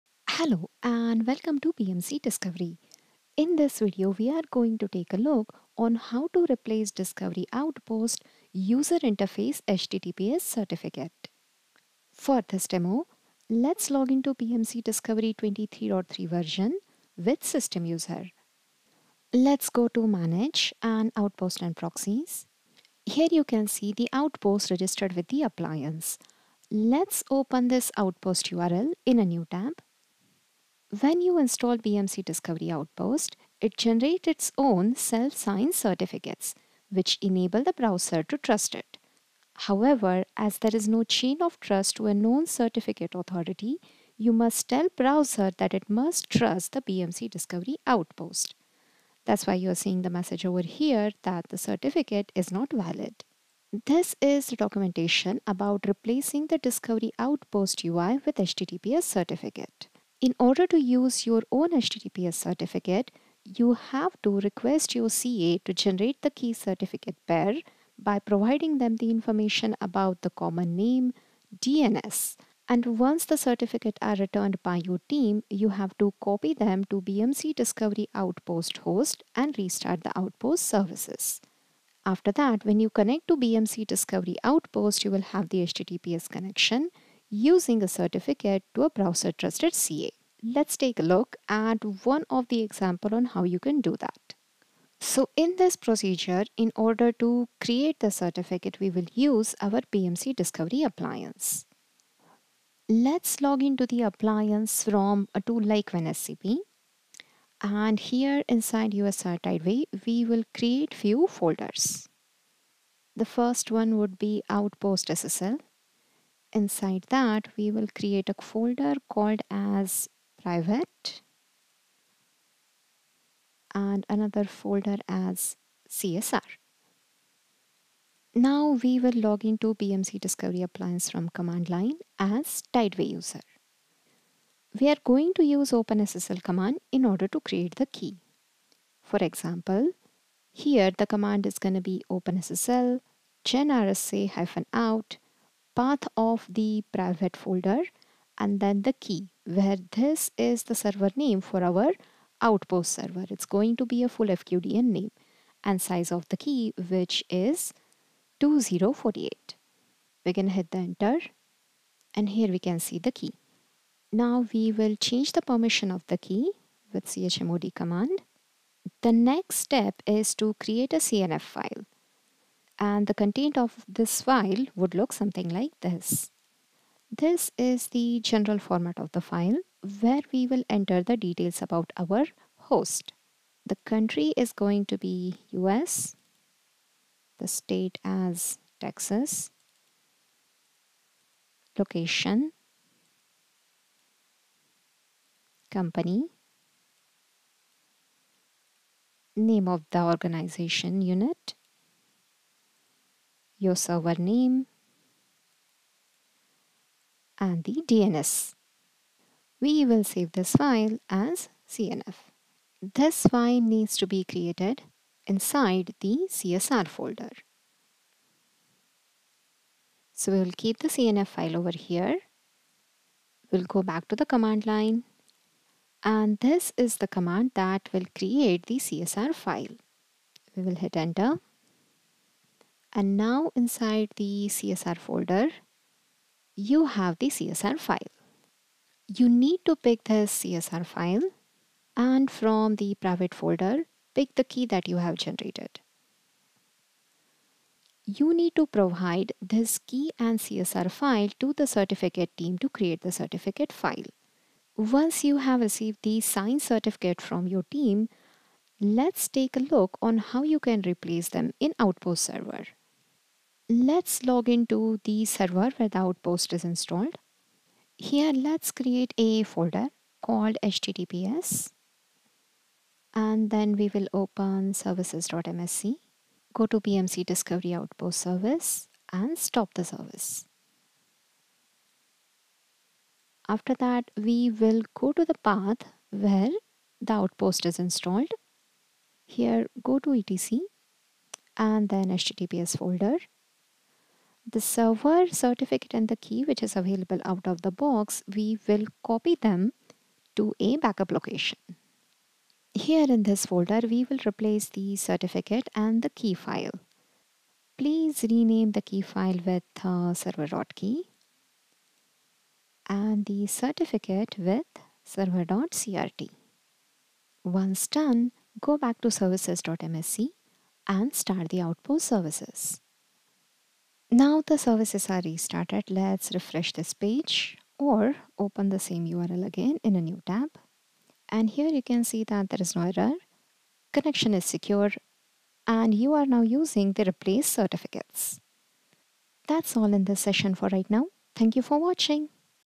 Helix Discovery: How to Replace Discovery Outpost UI HTTPS Certificate
In this video we will show you to replace Discovery Outpost UI HTTPS Certificate